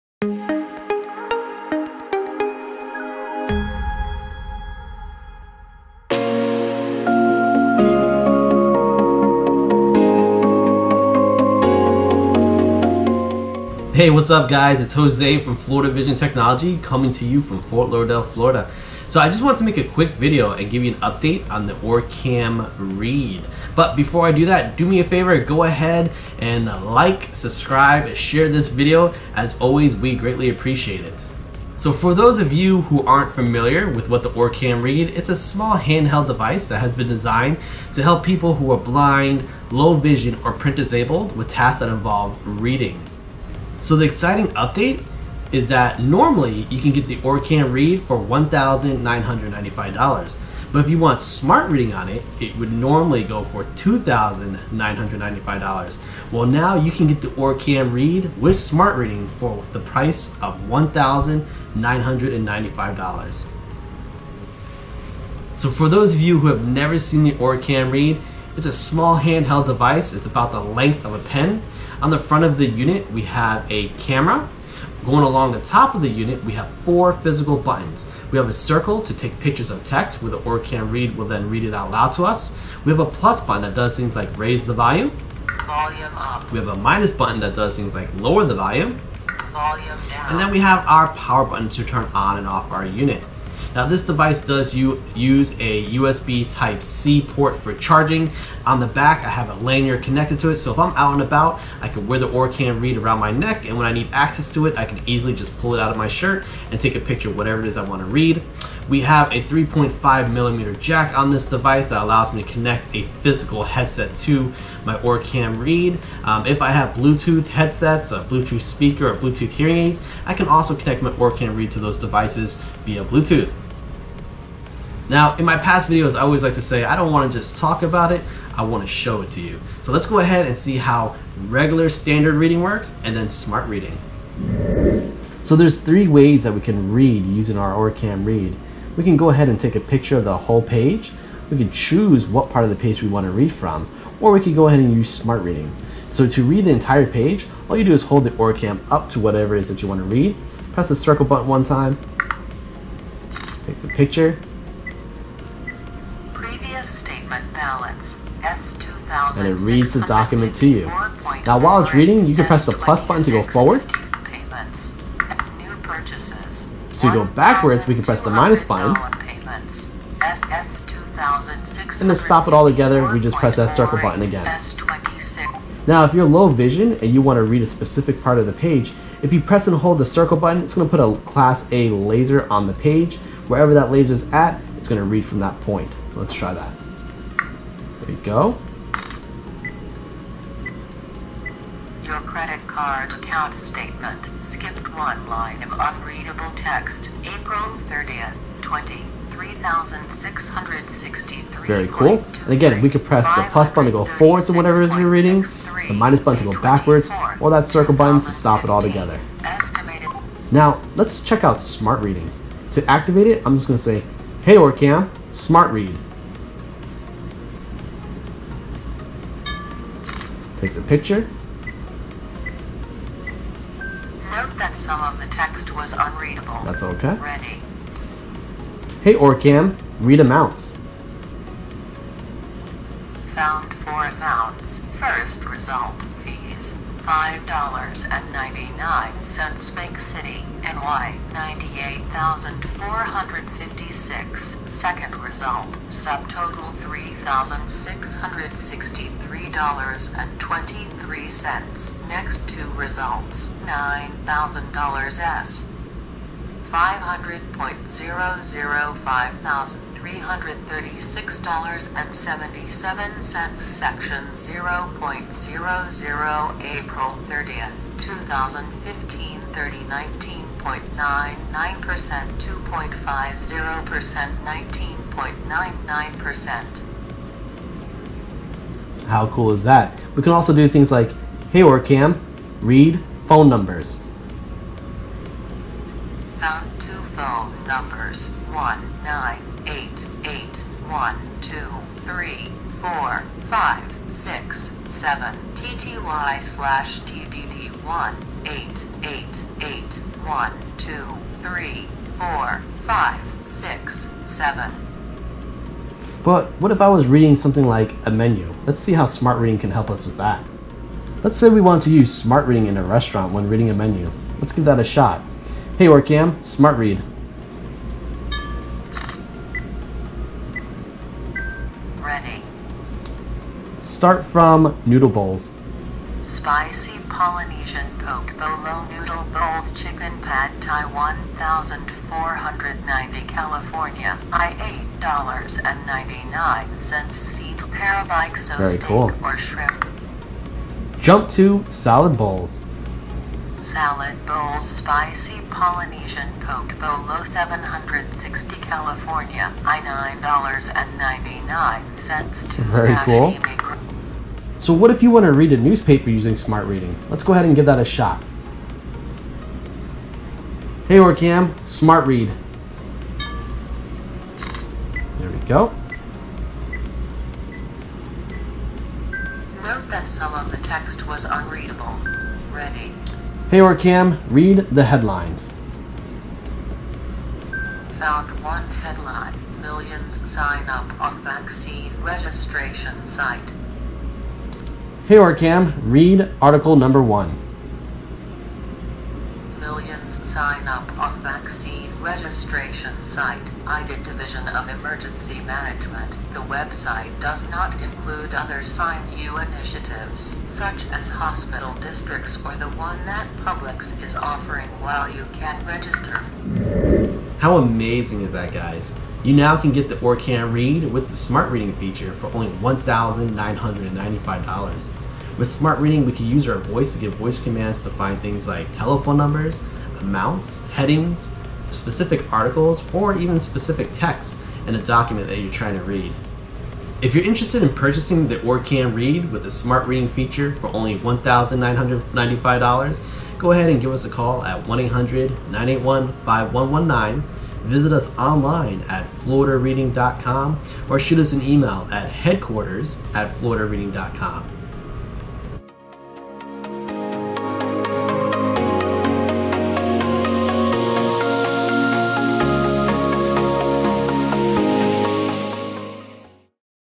Smart Reader Update For OrCam Read
We have exciting news involving the OrCam Read and its Smart Reading feature! Come find out what the news is and see how someone who is totally blind, low vision or print disabled can utilize the OrCam Read to independently perform various tasks throughout their day that require reading.

The OrCam Read is a handheld device that assists people that are print disabled, low vision or totally blind with tasks that involve reading. By combining a camera with artificial intelligence, users have instant access to printed materials for reading. Now users of the OrCam read can use their voice to perform voice commands to control their OrCam Reads when reading in school, at work, in a restaurant or just in the privacy of their homes.